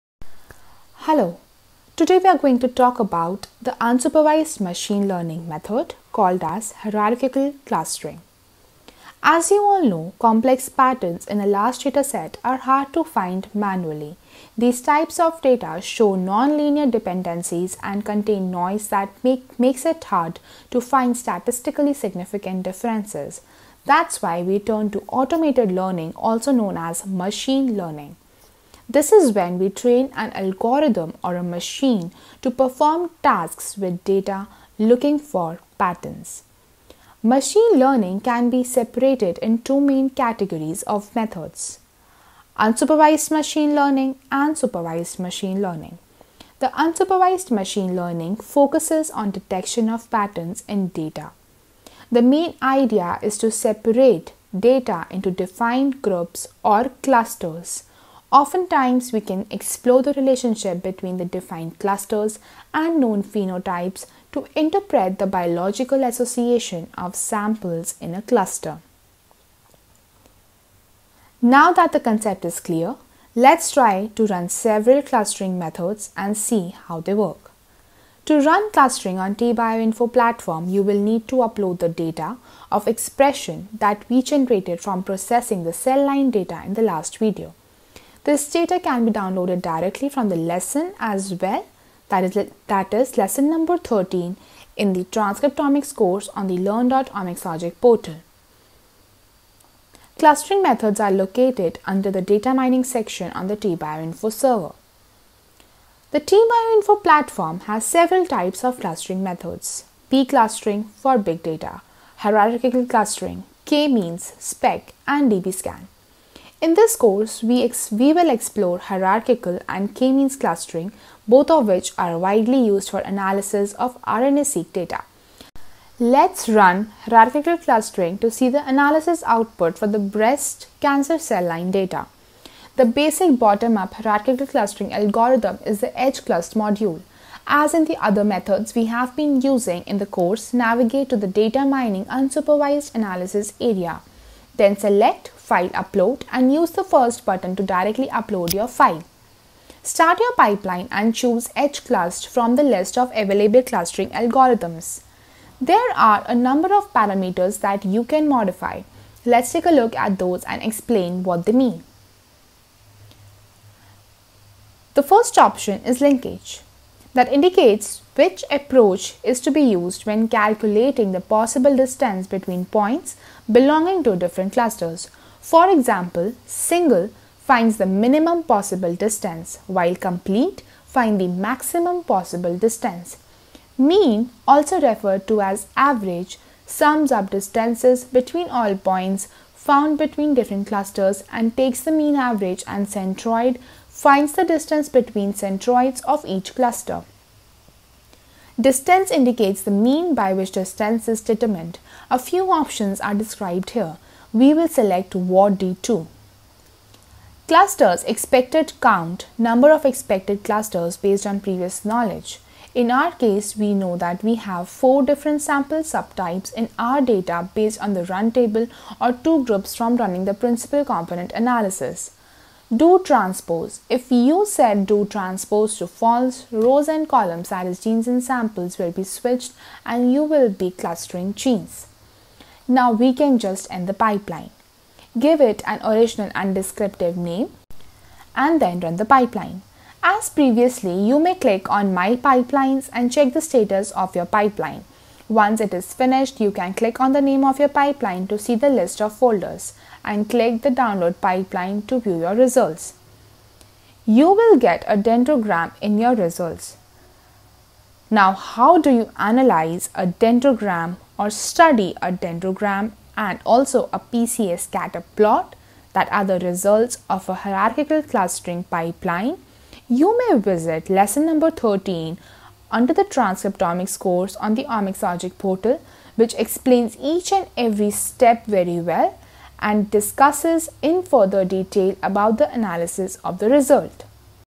Unsupervised Machine Learning (Hierarchical Clustering)
Complex patterns in large datasets are hard to find manually. These types of data show non-linear dependencies and contain noise that makes it hard to find statistically significant differences. That’s why we turn to automated learning, also known as “machine learning”. This is when we train an algorithm, or a “machine” to perform tasks with data, looking for patterns. The T-BioInfo platform has several types of clustering methods: P-clustering for big data, hierarchical clustering (H-Clust), K-means, Specc and DBScan.
 The basic, bottom-up hierarchical clustering algorithm is the H-clust module. As in the other methods we have been using in the course, navigate to the Data Mining Unsupervised Analysis area.

To learn more about the various clustering algorithms, the outputs they generate and the analysis,

In this course, we will explore hierarchical and K-means clustering, both of which are widely used for analysis of RNA-seq data. After the completion of the pipeline, you would obtain a dendrogram in which you would be able to look at the clusters, distances between clusters and the order of samples.